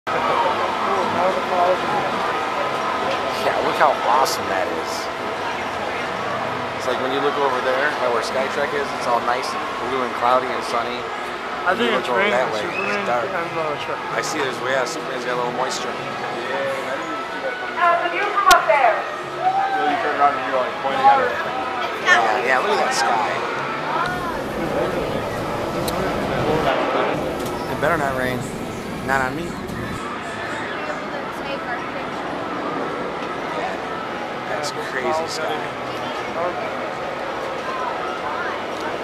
Condor and Clouds Six Flags Great America 7-31-13
The sky was coming in on us, and we decided to ride Condor! The sky was really nasty to the north! And it was coming south at us!